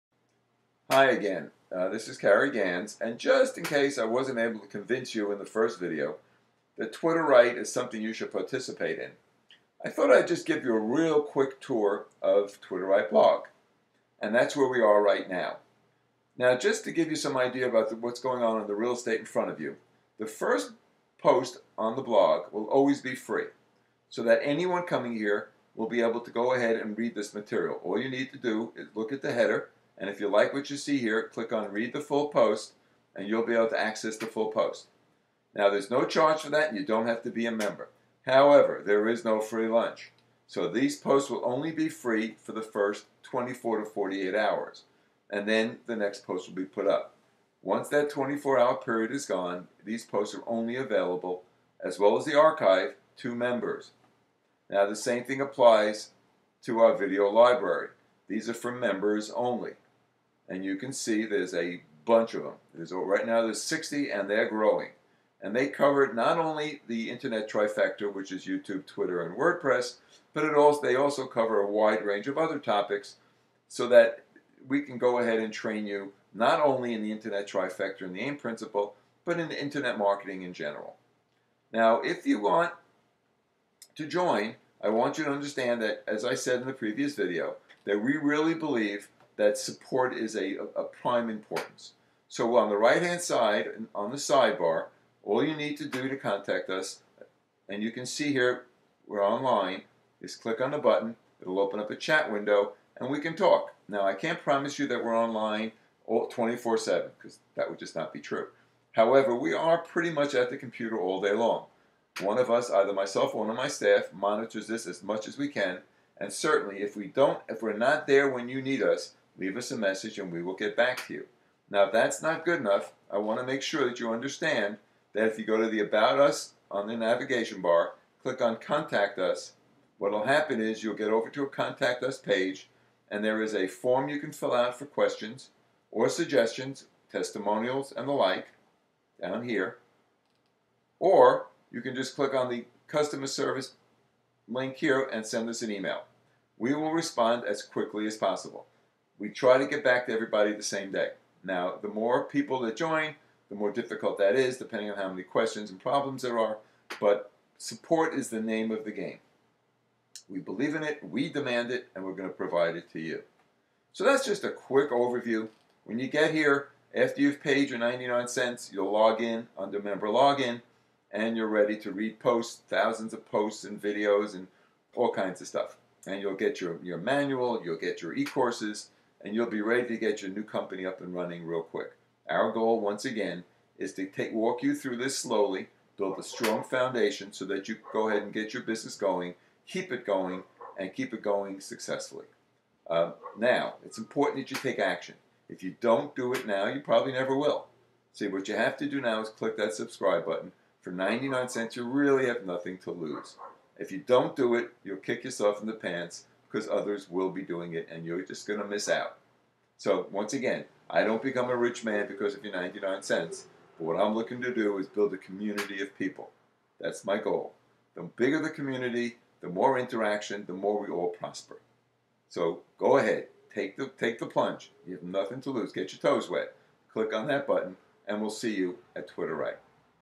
TwitterRight - Automating, Integrating and Monetizing WordPress, Twitter and YouTube
If you've ever wanted to start an Internet Business but felt it was either beyond your capability or, possibly more importantly, too expensive than you need to listen to this brief video! Learn how to master WordPress, Twitter and YouTube to create a successful online business. I promise it will be worth your time!